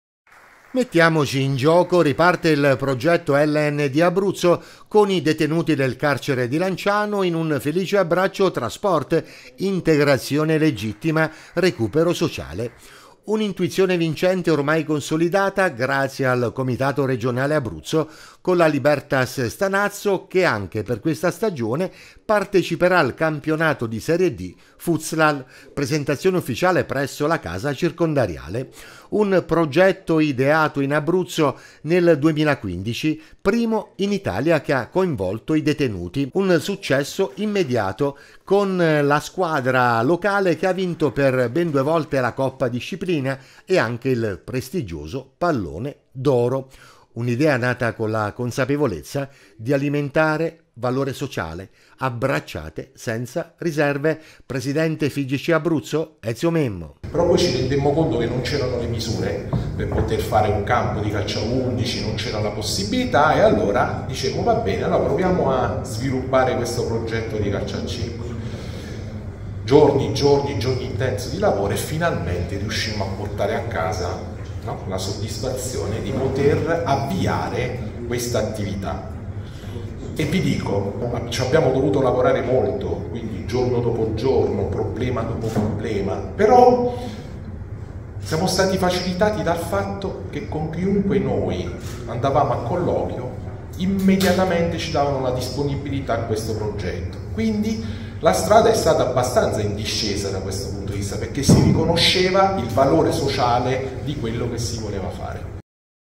Calcio a 5, detenuti sempre in gioco
"Mettiamoci in gioco": riparte il progetto della Lega Nazionale Dilettanti che mette insieme sport e sociale, con i detenuti del carcere di Lanciano che prenderanno parte al campionato Futsal di Serie D.